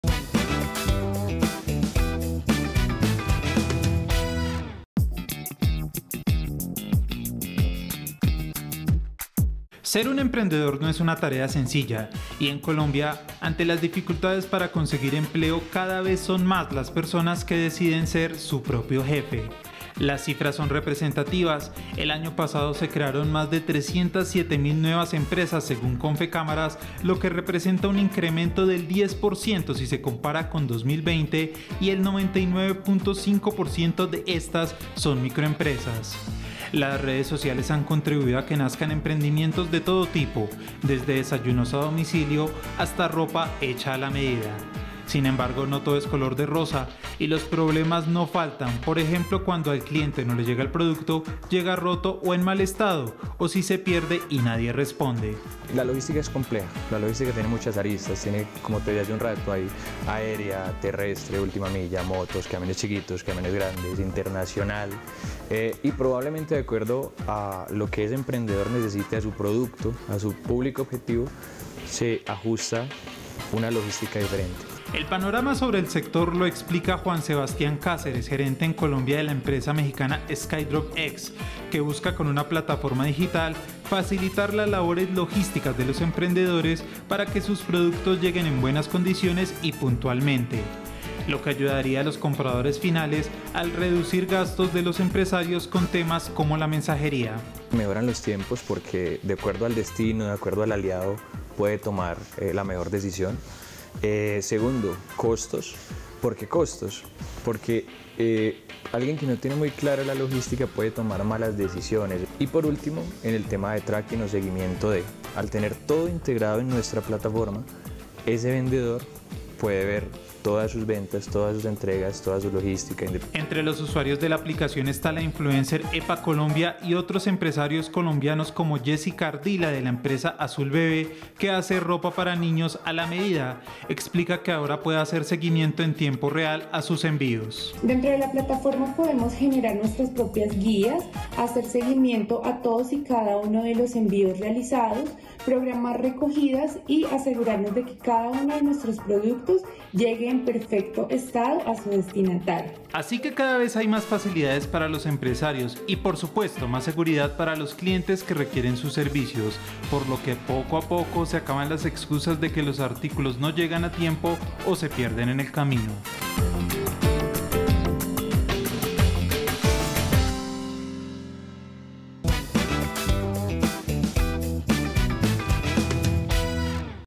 El mundo de los emprendedores y los retos en la logística en sus negocios I Impacto Económico
Ser un emprendedor no es una tarea sencilla, y en Colombia, ante las dificultades para conseguir empleo cada vez son más, las personas que deciden ser su propio jefe.

Las cifras son representativas: el año pasado se crearon más de 307 mil nuevas empresas, según Confecámaras, lo que representa un incremento del 10 % si se compara con 2020, y el  99, 5 % de éstas son microempresas.

Sin embargo, no todo es color de rosa, y los problemas no faltan,por ejemplo  cuando al cliente no le llega el producto, llega roto o en mal estado, o se pierde y nadie responde.